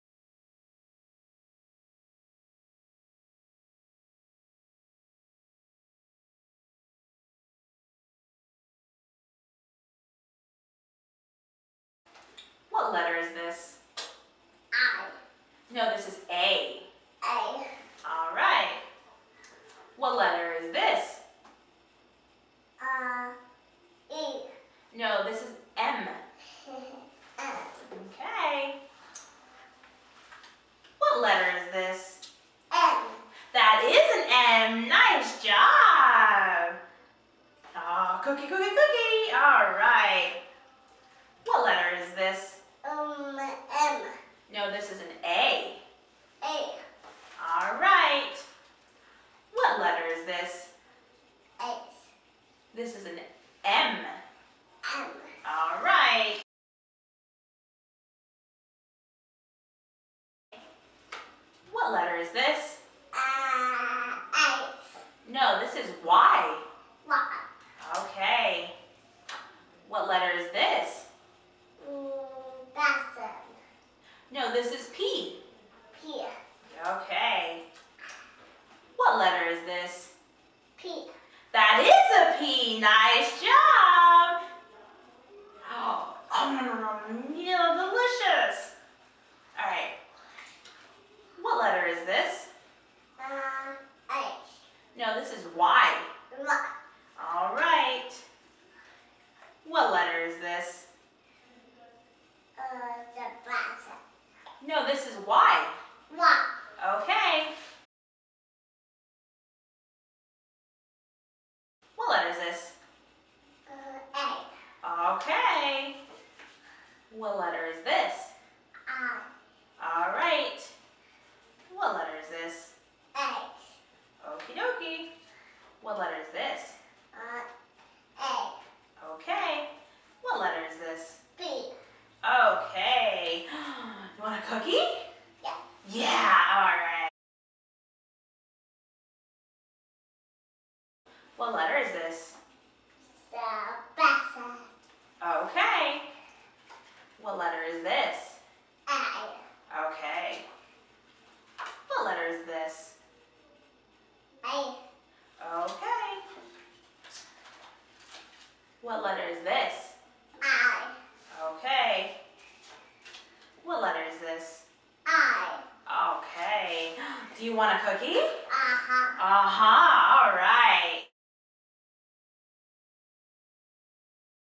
Serial and Concurrent Training Video Methods (Wunderlich et al., 2014)
Video examples of procedures described in Wunderlich et al. (2014).

Effects of serial and concurrent training on acquisition and generalization.
Kara L. Wunderlich, Timothy R. Vollmer, Jeanne M. Donaldson, & Cara L. Phillips
Journal of Applied Behavior Analysis, 47(4).
Winter 2014